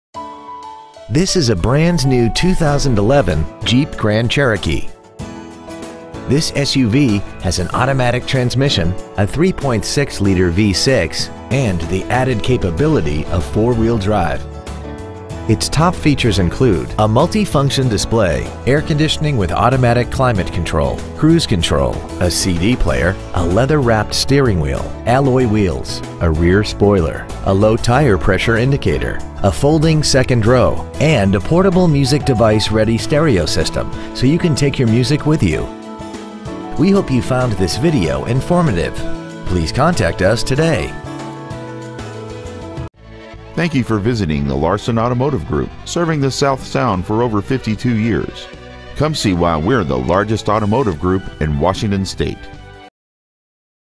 2011 Jeep Grand Cherokee Puyallup WA
Year : 2011

 Make : Jeep

 Model : Grand Cherokee

 Engine : 3.6L V6

 Trans . : Automatic

 Exterior : Mineral Gray

 Miles : 6

 Interior : Black

 Stock : 615344

 2001 N Meridian

 2011 Jeep Grand Cherokee Puyallup WA